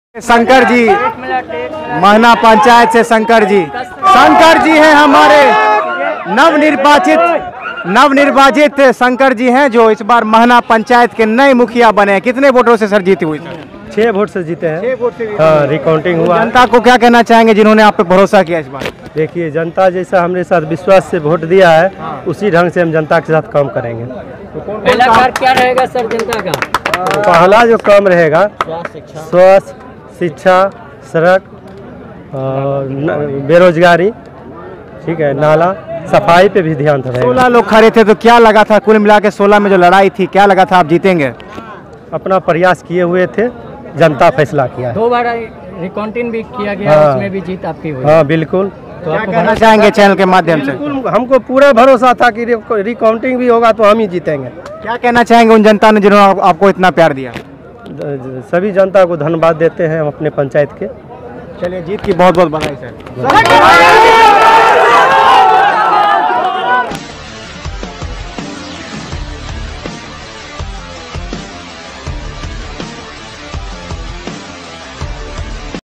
मात्र 6 वोट से जीते शंकर पंडित इस बार महाना पंचायत के नवनिर्वाचित मुखिया बनकर जनता ने उन्हें आगे लाय
मात्र 6 वोट से जीते शंकर पंडित इस बार महाना पंचायत के नवनिर्वाचित मुखिया बनकर जनता ने उन्हें आगे लाया है और विजई बनाया है